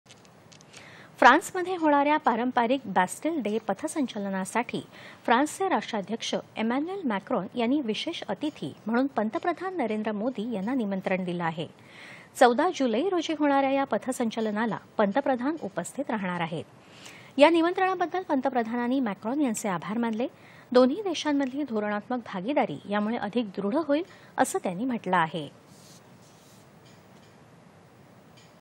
फ्रान्समध्ये पारंपरिक बॅस्टील डे पथसंचलनाचं आयोजन
दि. ०६.०५.२०२३ :
 फ्रान्समध्ये होणाऱ्या पारंपरिक बॅस्टील डे पथसंचलनासाठी फ्रान्सचे राष्ट्राध्यक्ष एमॅन्युअल मॅक्रॉन यांनी विशेष अतिथी म्हणून पंतप्रधान नरेंद्र मोदी यांना निमंत्रण दिलं आहे.   १४ जुलै रोजी होणाऱ्या या पथसंचलनाला पंतप्रधान उपस्थित राहणार आहेत.  या निमंत्रणाबद्दल पंतप्रधानांनी मॅक्रॉन यांचे आभार मानले. दोन्ही देशांमधली धोरणात्मक भागीदारी यामुळे अधिक दृढ होईल, असं त्यांनी म्हटलं आहे.